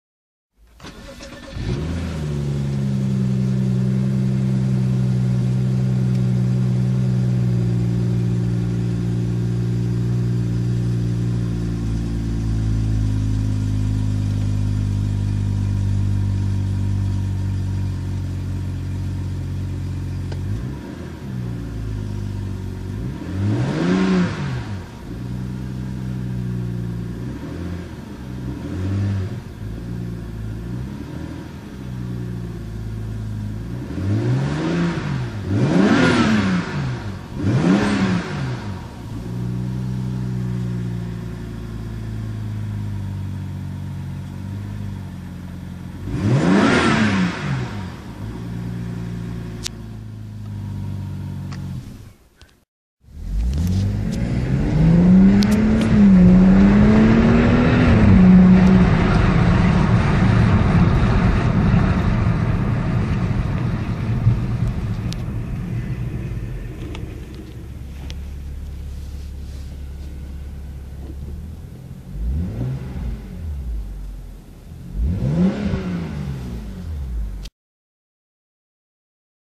BMW 335i Start Up & Revs with Fast Acceleration
BMW 335i 3.0 6-cilinder BiTurbo
306hp, 400Nm
0-100 km/h: 5,5 sec

@ Holtum, The Netherlands